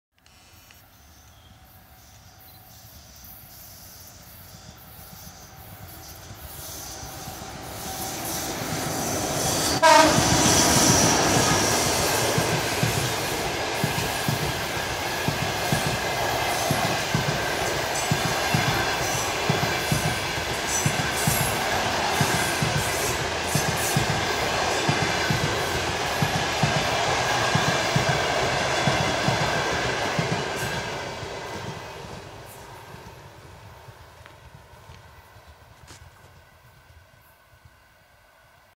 чс4-052 з "ПОДIЛЬСЬКИЙ ЕКСПРЕС" № 769 Kиїв-Камянець-Подільський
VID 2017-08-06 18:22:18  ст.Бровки

«ПОДольсикй экспресс» № 769 Kиев-Камянец-Подольский с группою вагонов до Могилев-Подольского